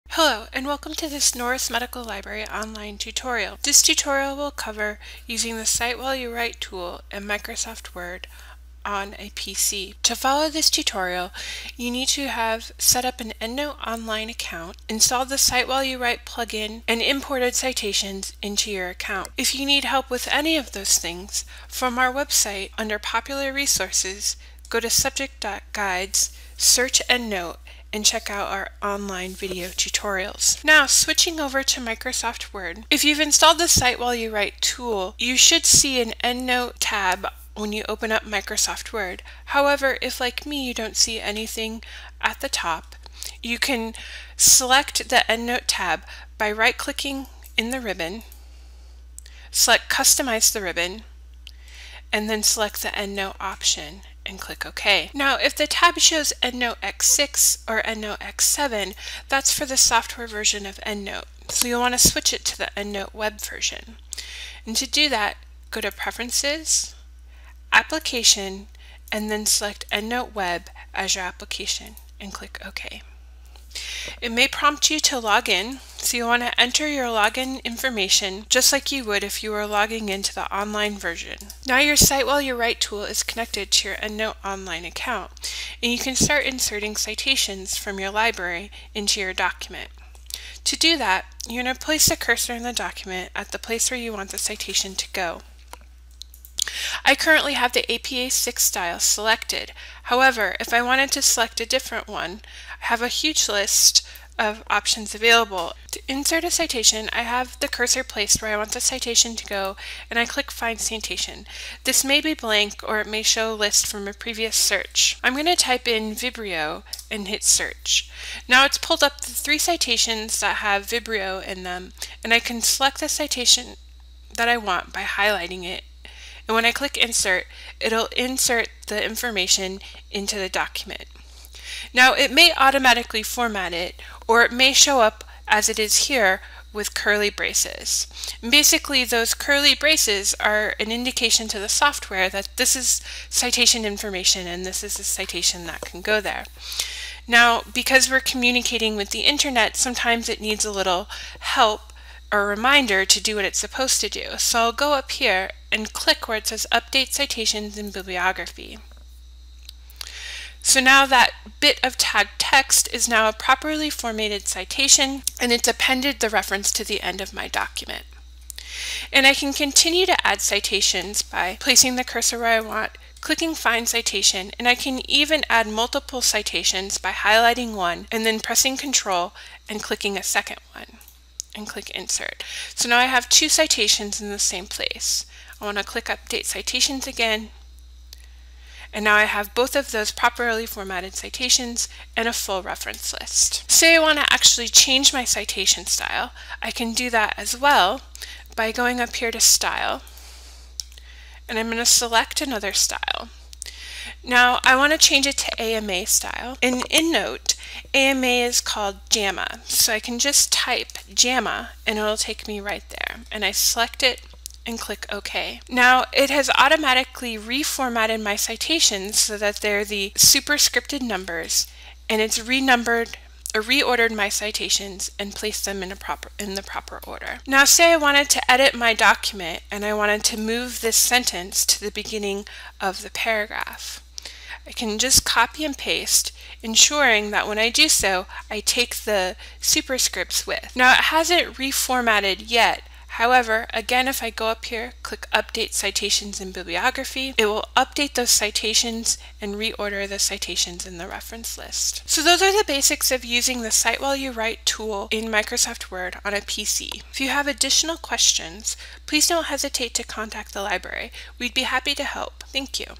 Using EndNote's Cite-While-You-Write tool on a PC
This tutorial from Norris Medical Library details how to use the Cite-While-You-Write tool in Microsoft Word on your PC.